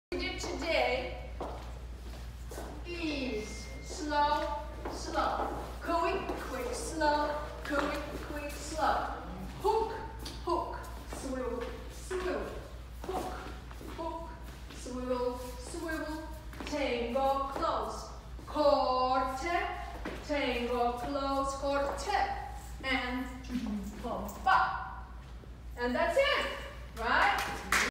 AEA Dance Club American Tango class 3
American Tango Class 3 taught by Irina Trebunskaya and Konstantin Yakunin of You Can Dance Studio.  This video demonstrates the reverse turn, corte as well as ganchos